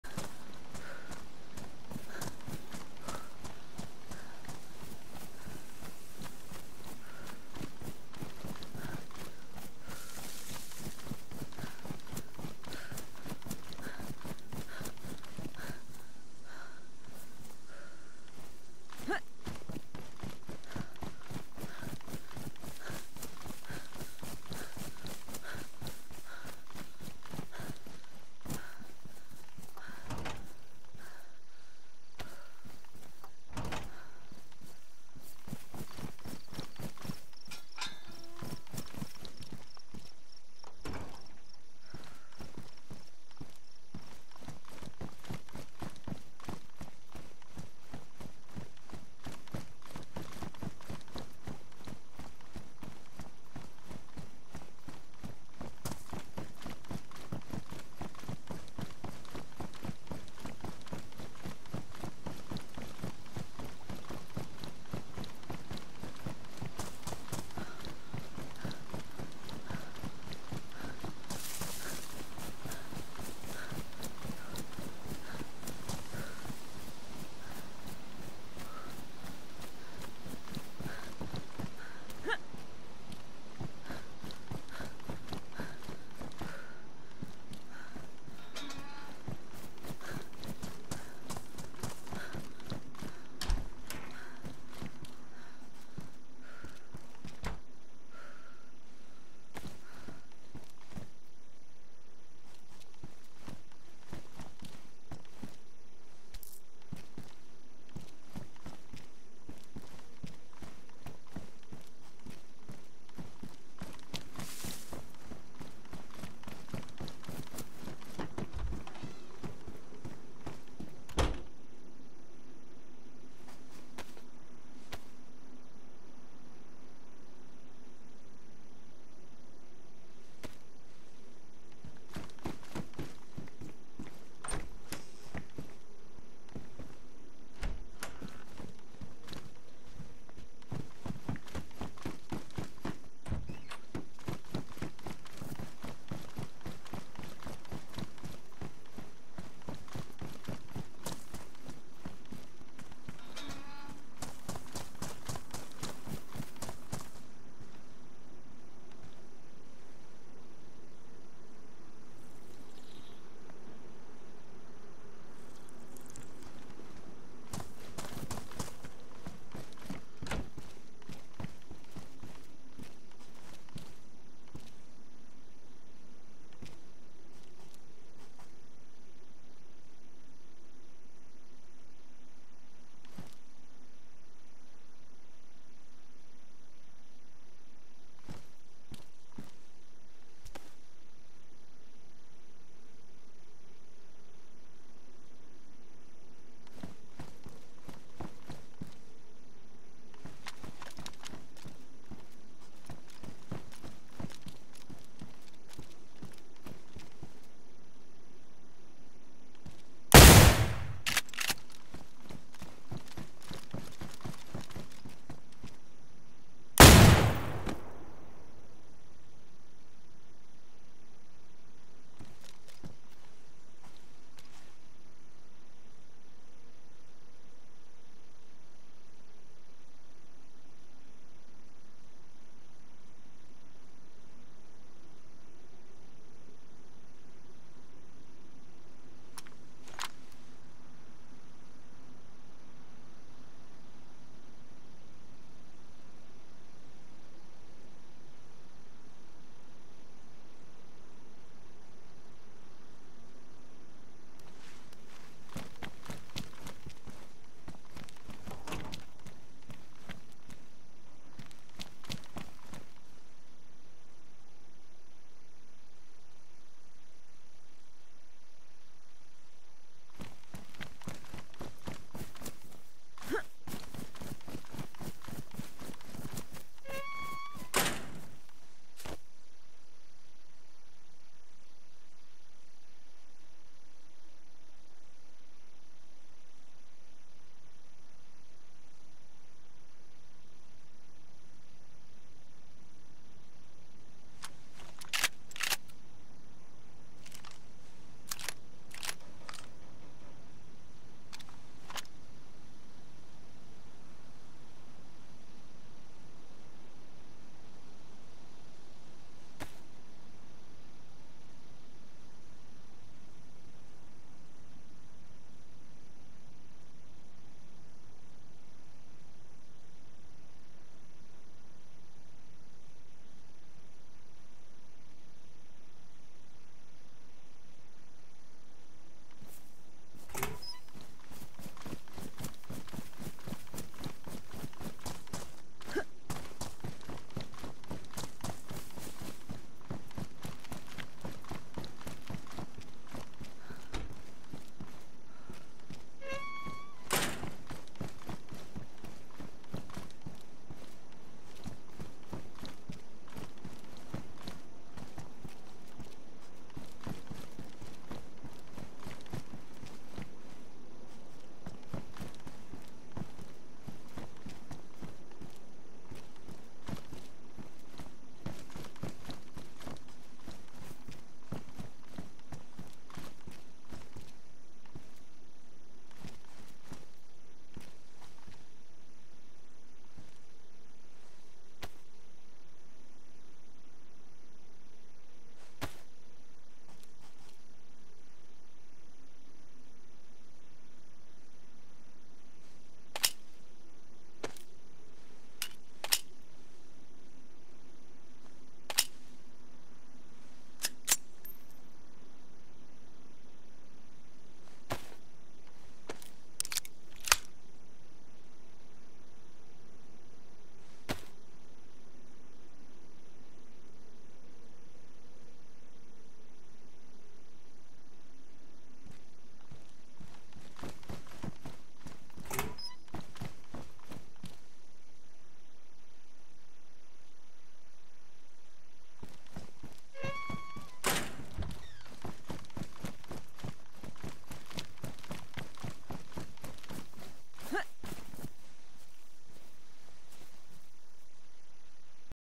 Stalking My Prey
In today's episode of Coltin being fucking dumb we watch as he stares at a dude for nearly 5 minutes not knowing what to do with him so he finally kills him and has trouble getting the loot off him but besides that I hope you all have a fantastic day and god bless you all!!!